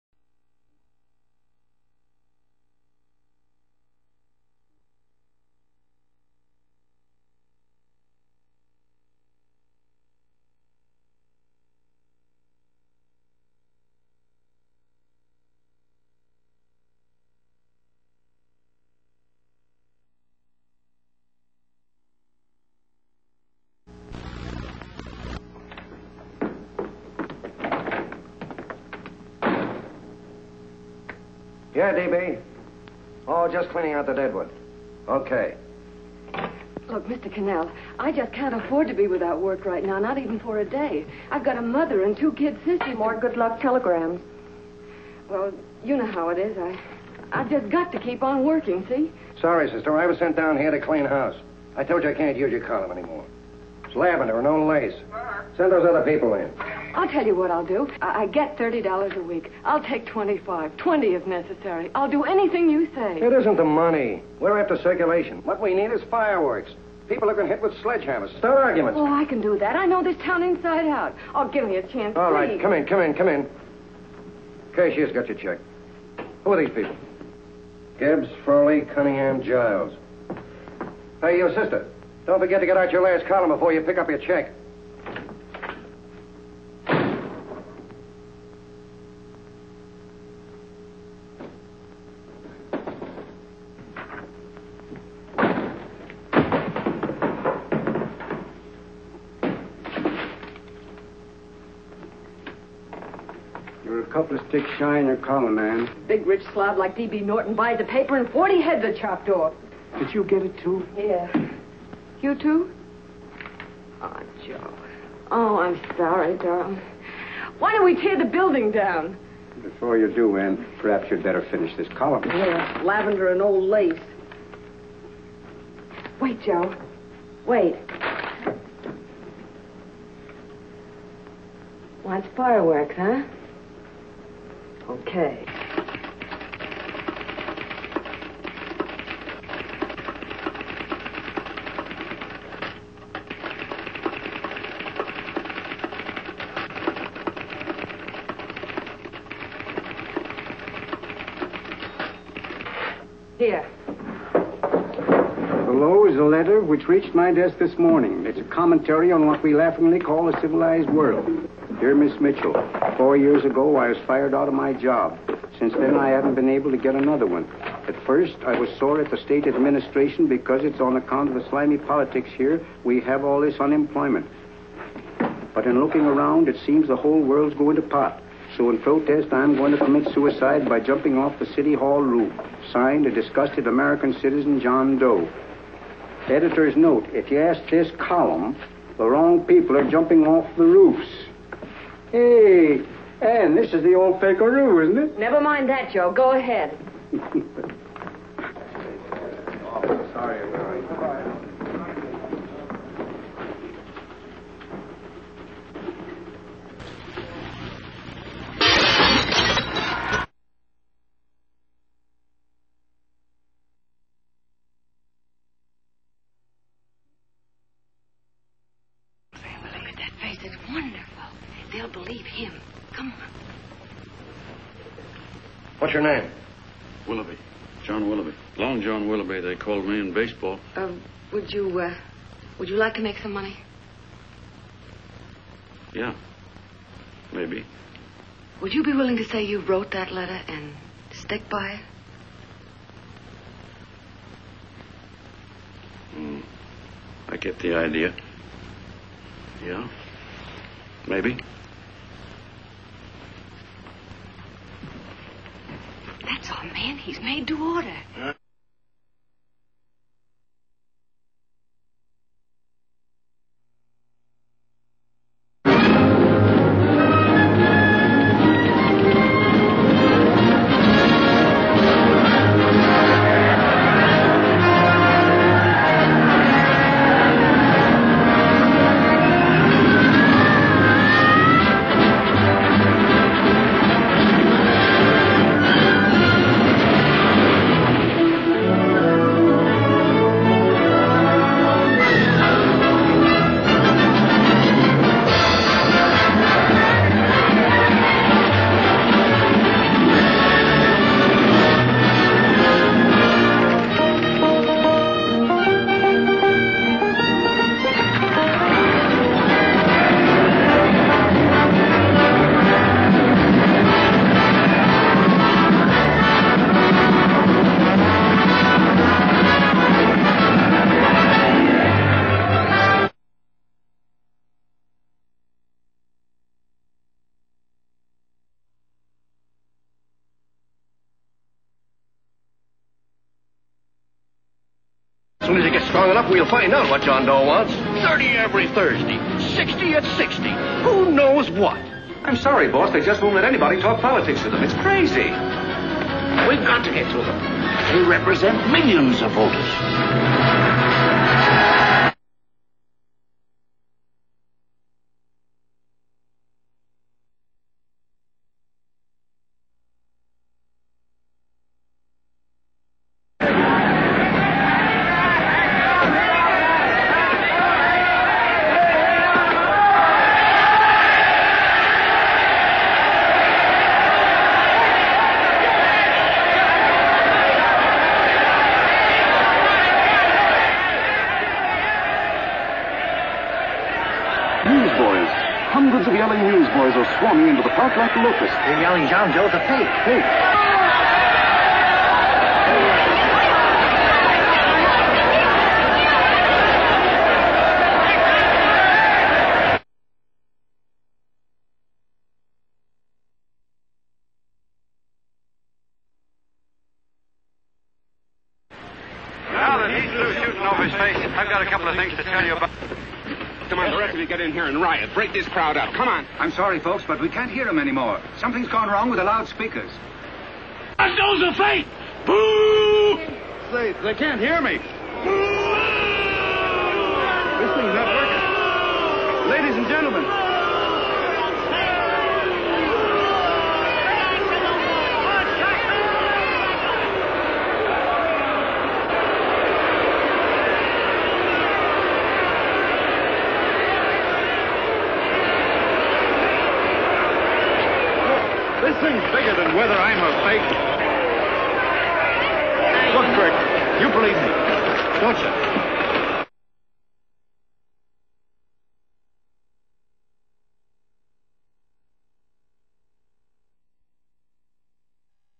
California Pensionite Movements Reflected in Movie
The film "Meet John Doe" appears to be based on California pensionite movements such as the Townsend Plan and Ham & Eggs.  These involved formation of clubs around the state (and nation in the case of Townsend) to support the movement.  Ham & Eggs involved a plan to pay every state resident over age 50 Thirty Dollars Every Thursday.  (The plan appeared on the ballot in 1938 and 1939.)  There is a vague reference in the film to this notion.  Moreover, the suicide theme in the film was based on an actual incident seized on by Ham & Eggs supporters.  For more info on California pensionites, see Mitchell, "Pensions, Politics, and the Elderly" (M.E. Sharpe, 2000).